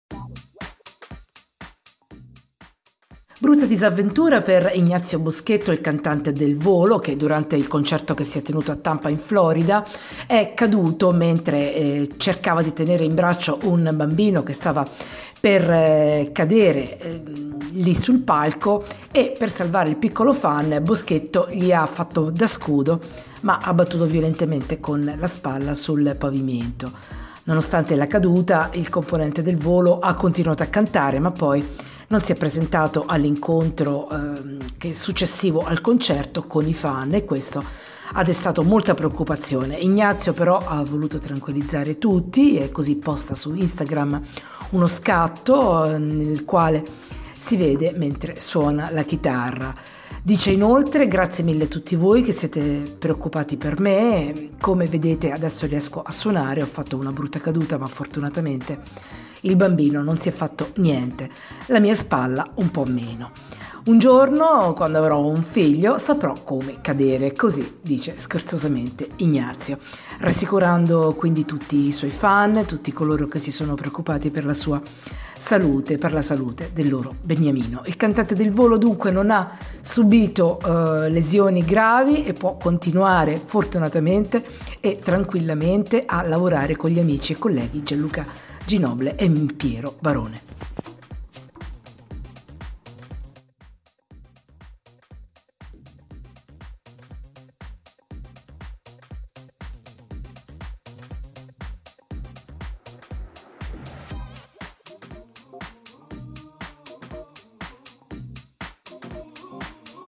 IL VOLO IGNAZIO BOSCHETTO BRUTTA CADUTA SUL PALCO
Ignazio Boschetto, La caduta dal palco preoccupa i fans.